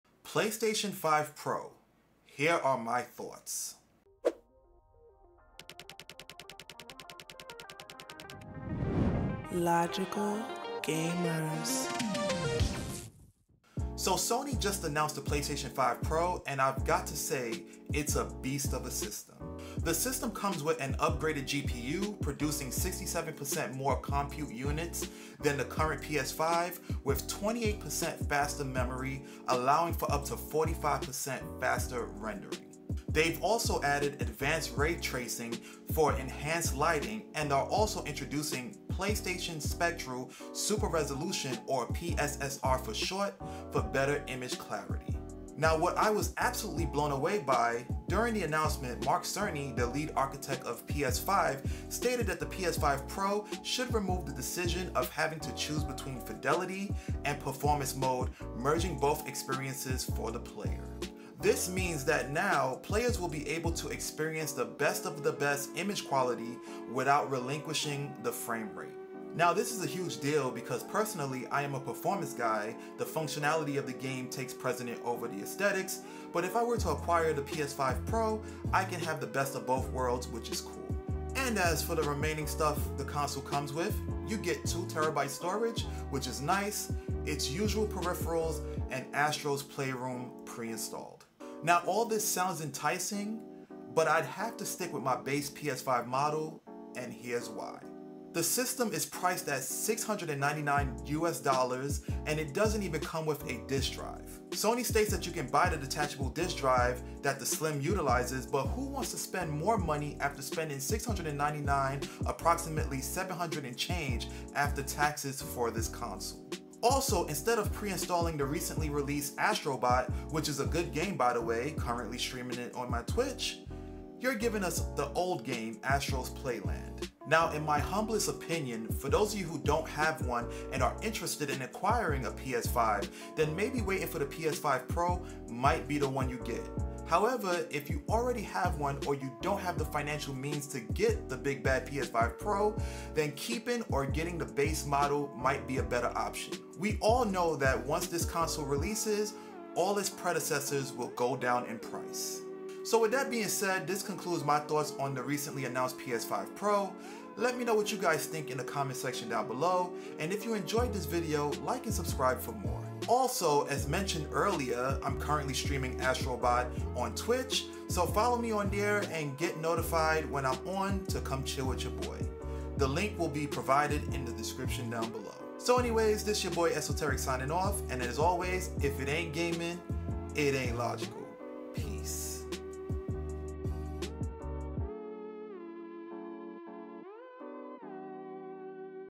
PS5 Pro... Is It Worth It?
Here are my thoughts on the newly announced PlayStation 5 Pro.